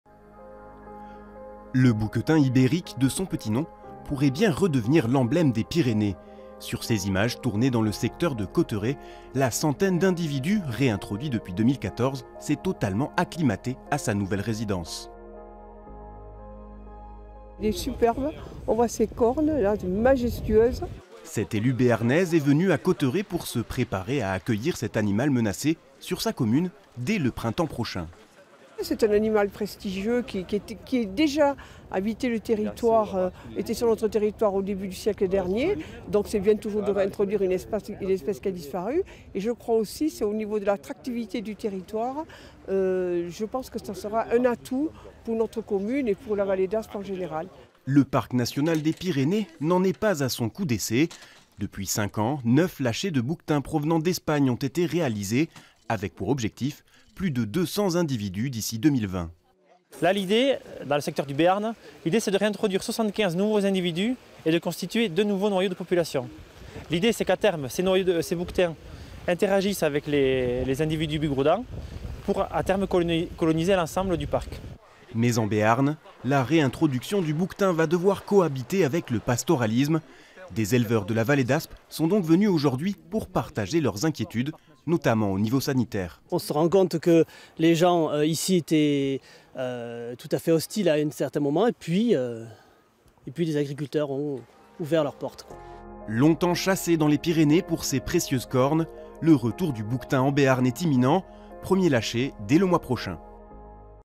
Bouquetins bientot en bearn
Après la réintroduction une centaine de bouquetins ibériques dans les Hautes-Pyrénées pour sauver cette espèce emblématique du massif, un premier lâcher aura lieu au printemps 2019 en Béarn.